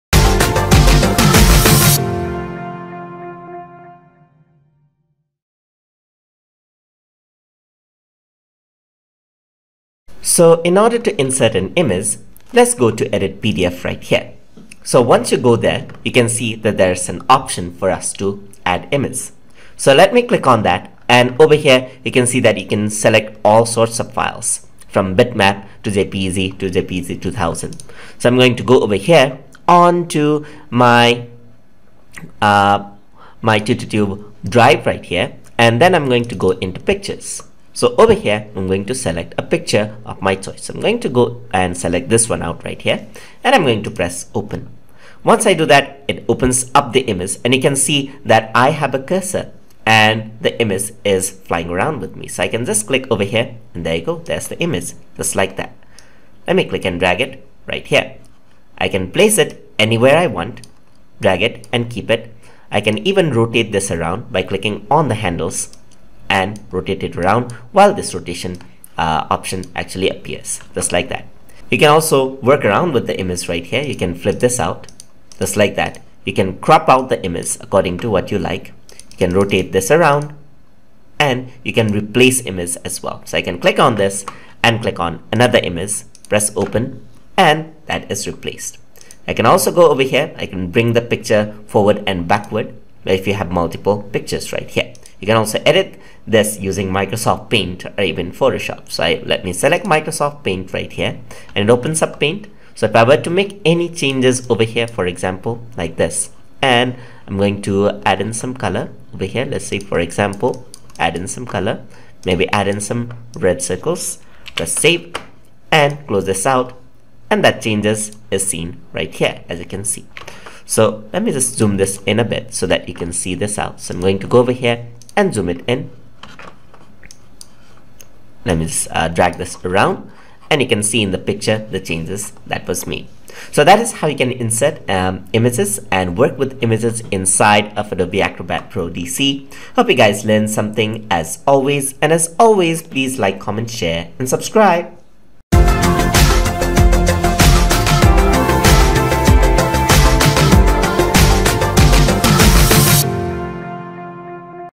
Adobe Acrobat Pro Tutorial - Lesson 39 - Inserting Pictures
In this tutorial, we will be discussing about Inserting Pictures in Adobe Acrobat Pro.

Learn the basics of using Adobe Acrobat Pro including how to work with PDF documents, modify doccuments, combine documents and make them interactive.

This lesson has been made using Adobe Acrobat Pro DC 2018 but is applicable for older and newer releases of the application as well.

If you like this video, here's our entire playlist of Adobe Spark tutorials: